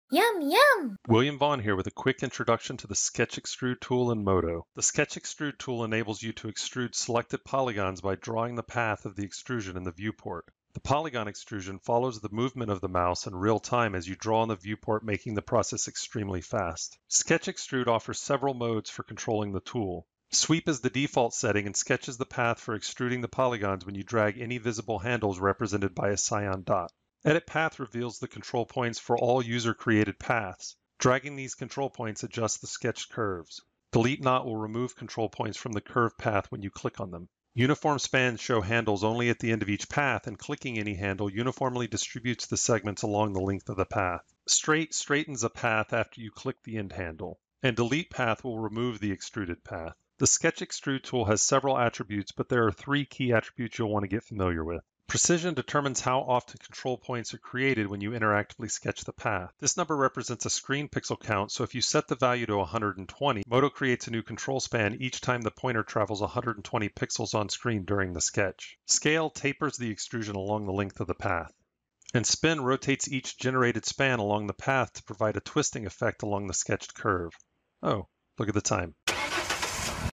MODO | Sketch Extrude
This video is a quick introduction to the Sketch Extrude Tool in MODO. The Sketch Extrude tool enables you to extrude selected polygons by drawing the path of the extrusion in the viewport. The polygon extrusion follows the movement of the mouse in real-time as you draw in the viewport making the process extremely fast.